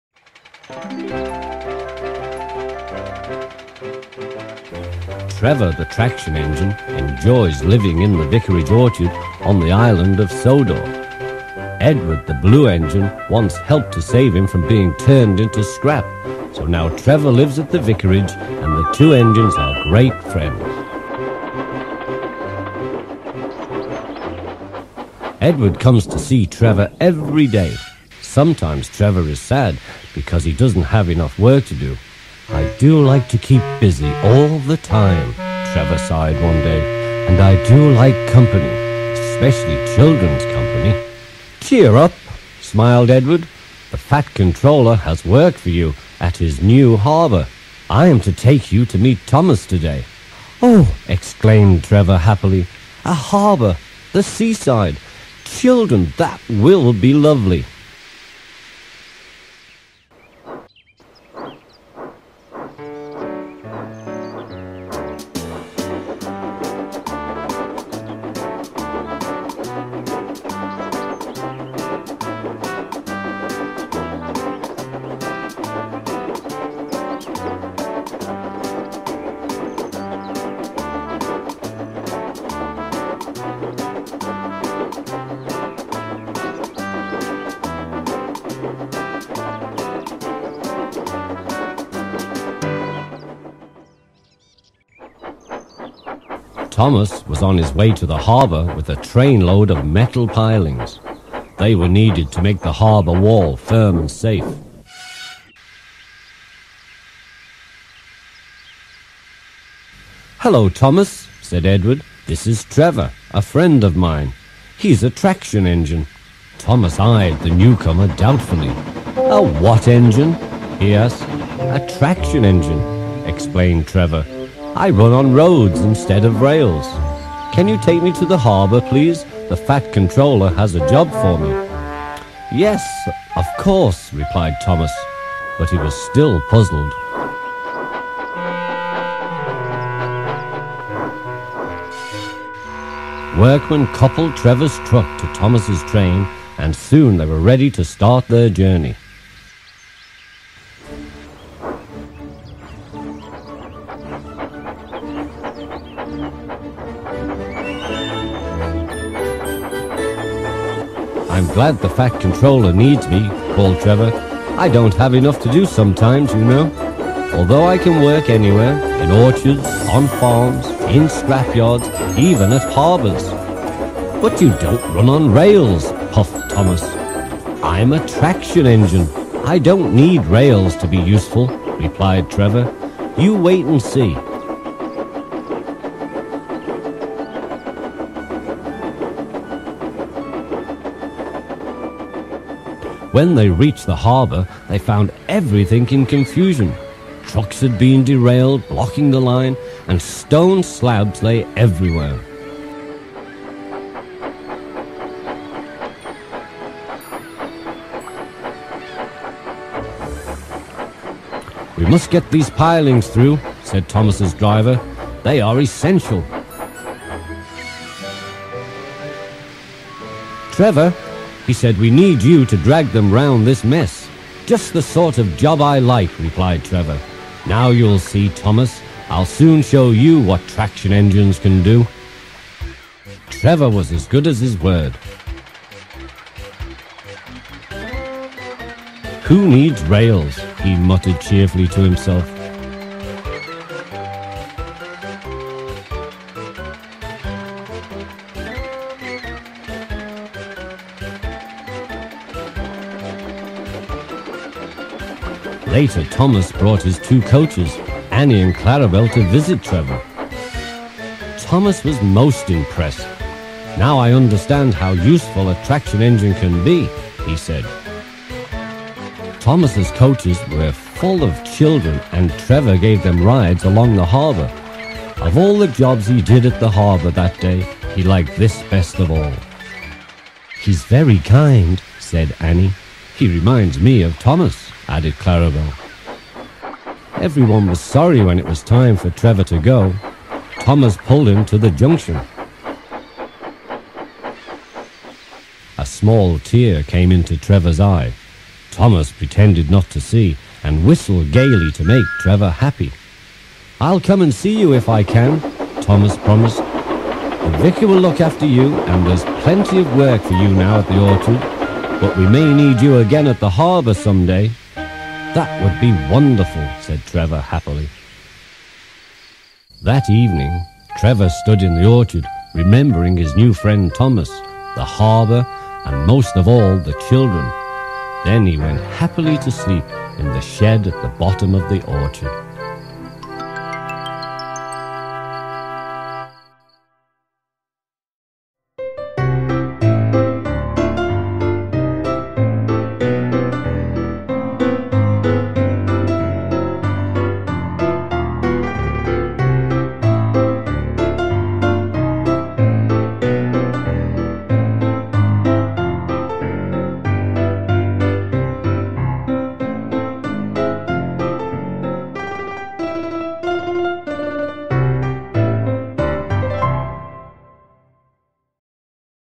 Thomas and Trevor
Tank Engine Thomas again is still in production but i wanted to do a Ladybird remake so here is one since this is a not a story from a book

Note:In some parts i forgot to place some sound effects and i only realized it after i renderd it and i can´t be bothered to re-render it also i forgot to credit carson08022000.for trevors whistle sorry bud

so anyway enjoy this little thing and keep an eye for Tank Engine Tomas Again